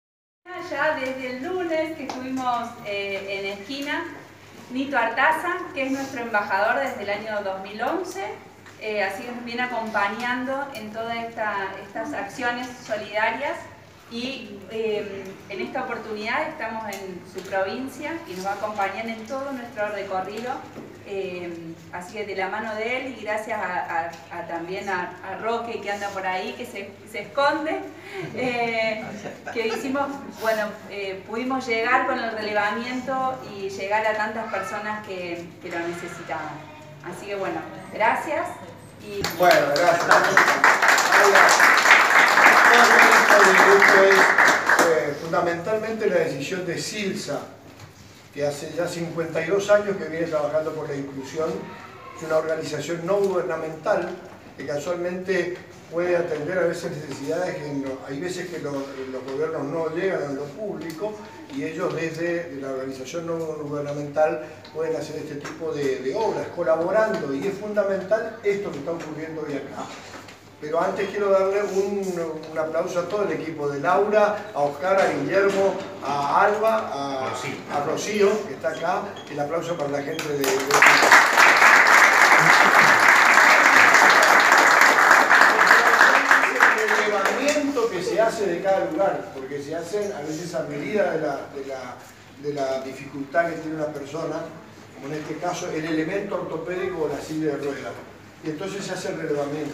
Nito Artaza en el CADI
El político y artista Eugenio Artaza visitó el CADI de nuestra ciudad como referente de CILSA.- Ituzaingó, Corrientes. 2018